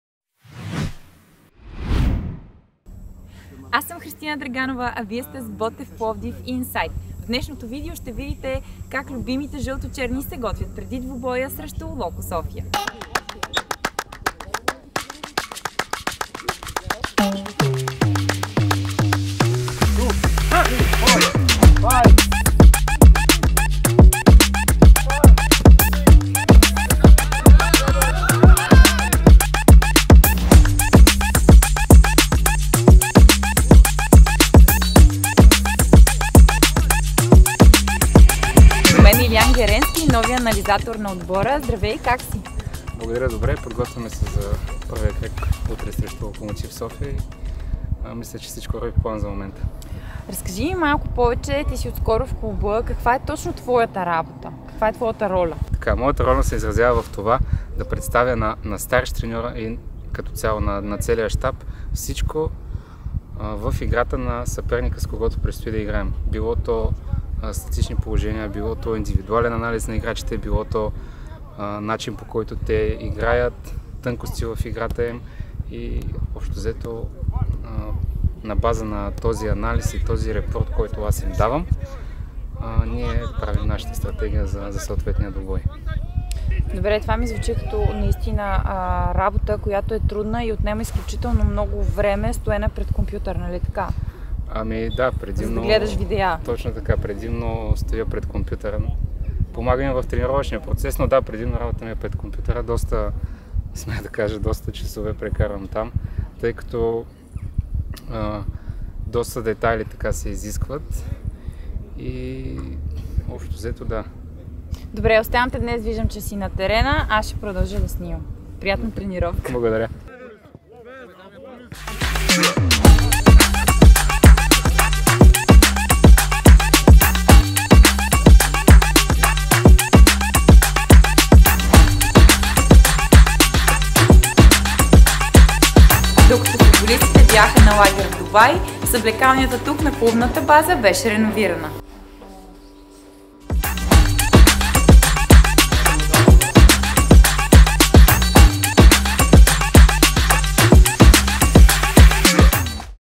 Botev Inside | Отборът се готви за мача с Локомотив (София)
Професионален футболен клуб Ботев Пловдив ©